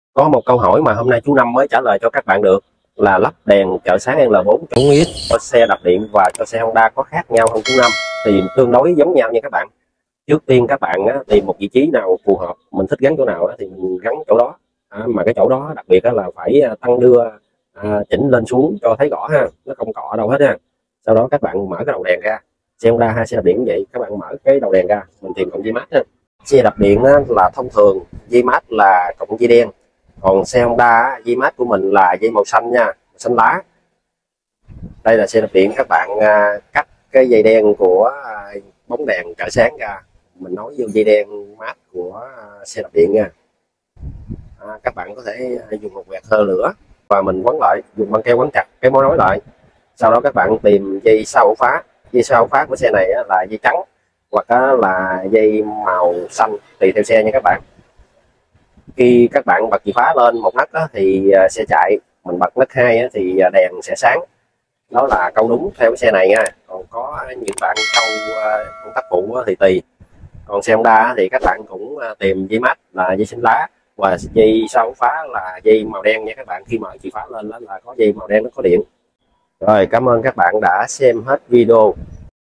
Hướng dẫn cách lắp đèn trợ sáng L4X cho xe đạp điện
Đây là cách lắp đèn trợ sáng L4X cho xe đạp điện và bạn có thể áp dụng lắp cho xe Honda luôn nha,cảm ơn bạn đã xem hết video này nhé